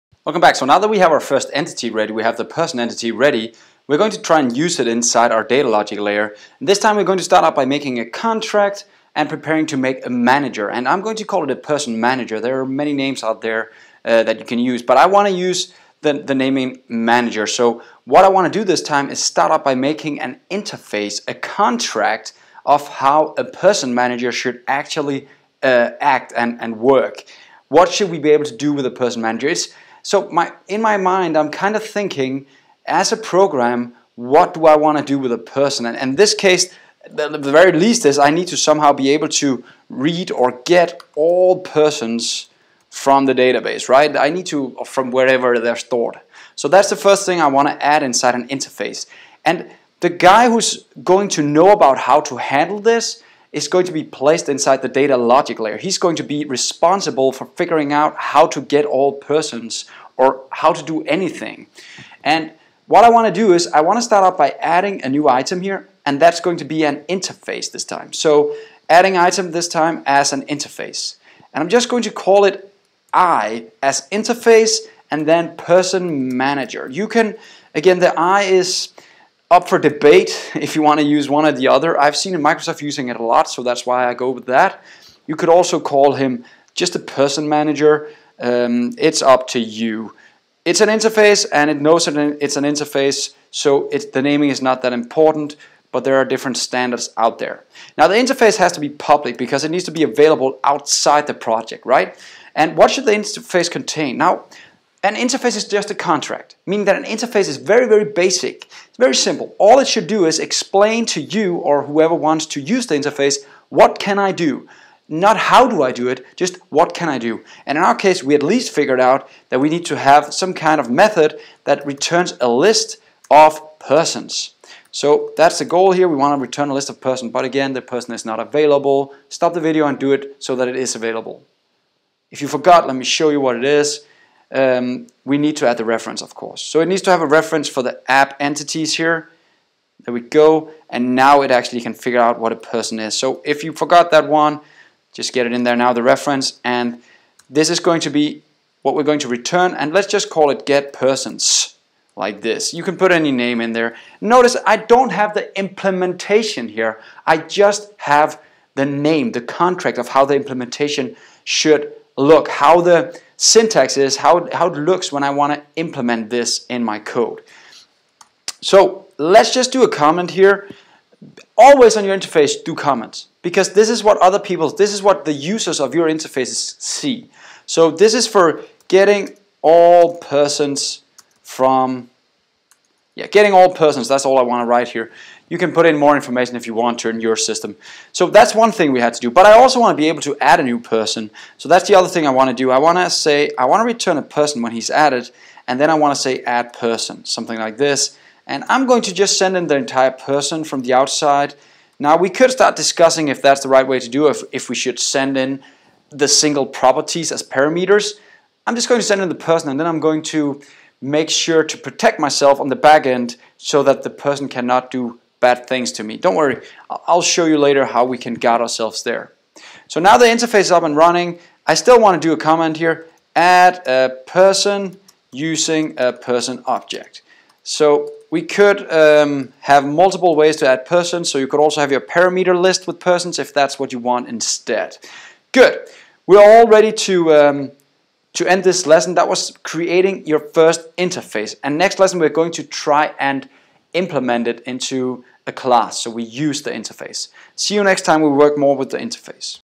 Data Logic Layer - Class Library or DLL file and first Interface
We will build Data Logic Layer Class Library that can be shared between projects.